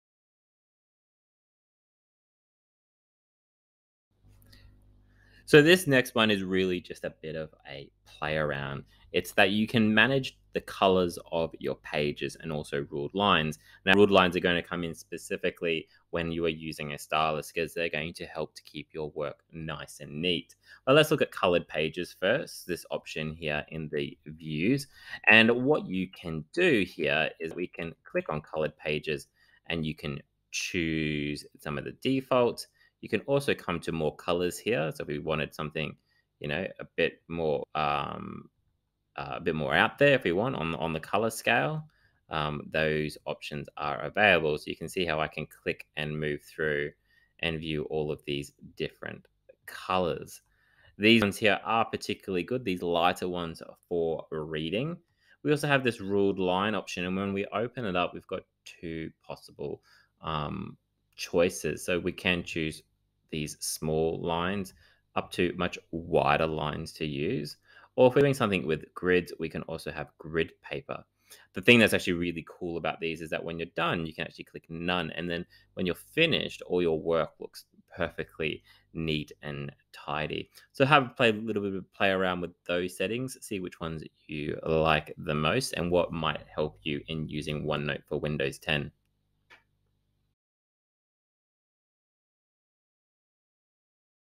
Class Notebook for Students - Coloured Page and Lines on Windows 10
Teach your students how to use the tools that OneNote for Windows 10 has to offer. Download a complete guide of our OneNote then distribute the pages to your students.